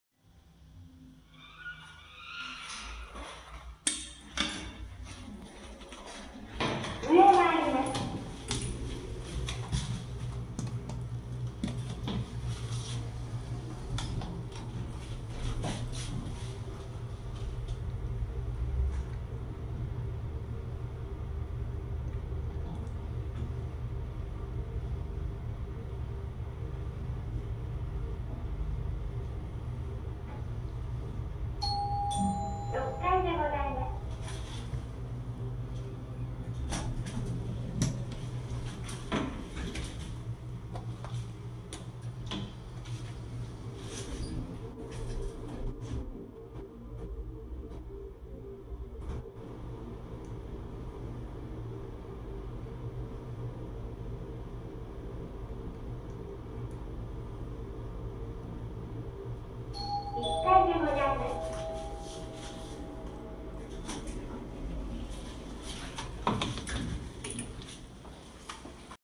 さがみ農協茅ヶ崎ビルのエレベーター
さがみ農協茅ヶ崎ビルのエレベーターです。
アストロからの更新で到着音はキンコンベルのままです。

データ
メーカー：日立
構造：ロープ式
モデル：アーバンエース
用途：乗用
定員：15名
積載：1000Kg
設置数：1
停止階：B,1,2,3,4,5,6
車椅子操作盤：あり
アナウンス：あり
チャイム・ベル：あり
ボタン確認音：なし